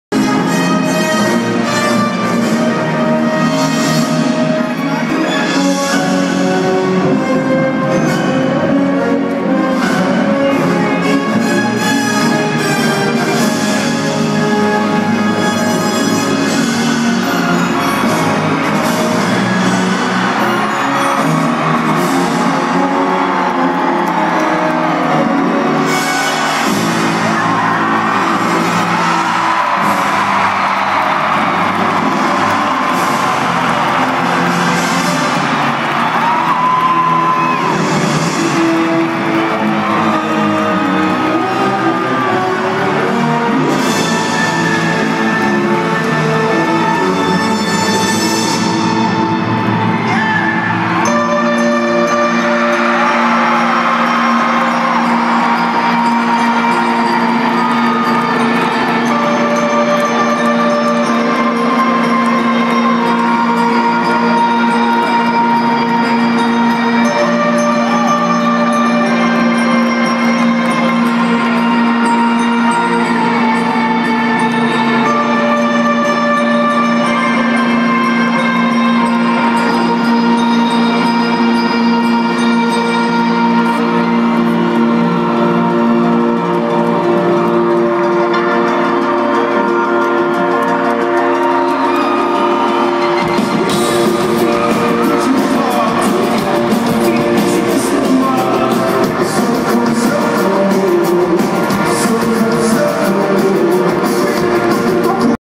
Opening at Coldplay Concert
Anticipating the arrival of the band, Coldplay. Listen to the excitement when they hit the stage, and everyone's bracelets light up in sync to the music.